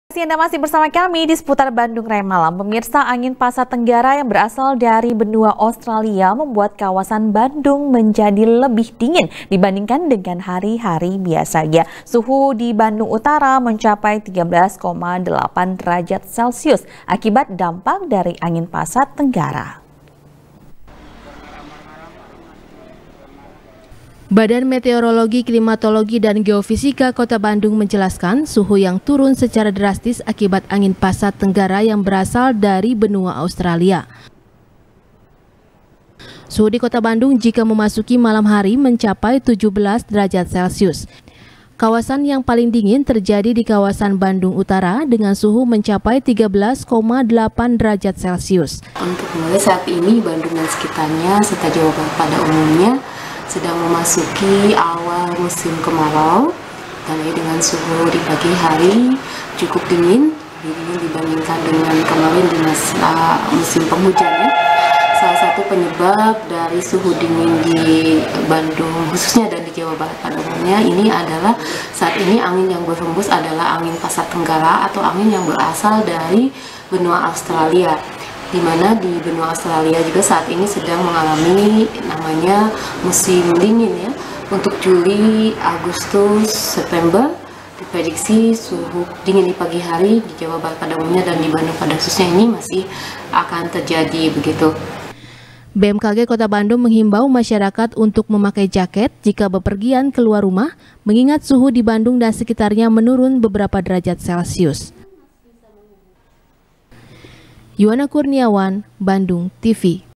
Cuaca Dingin Bandung Akibat Angin Pasat Tenggara Australia | SBR | BANDUNGTV
SBR | BANDUNGTV | KRISANTI | Angin Pasat Tenggara,yang berasal dari Benua Australia,membuat kawasan Bandung menjadi lebih dingin dibandingkan dengan hari-hari biasanya. Suhu di Bandung Utara,mencapai 13 koma delapan derajat celcius,akibat dampak dari Angin Pasat Tenggara.